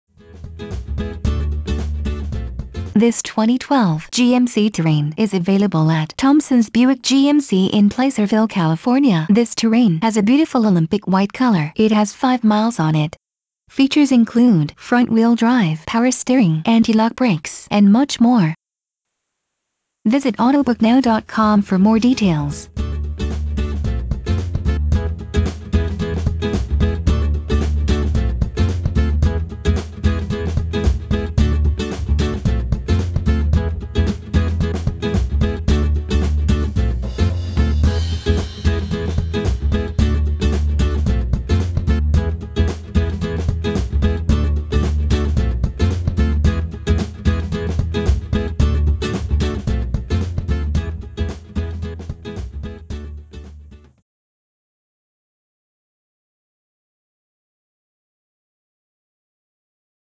2012 GMC Terrain SLT-1 in Placerville CA for Sale
2012 GMC Terrain SLT-1
Stock #: G12155
Price: $30,016
Miles: 5
Trim: SLT-1
Color: Olympic White
Interior: Jet Black
Transmission: Automatic
Drivetrain: FWD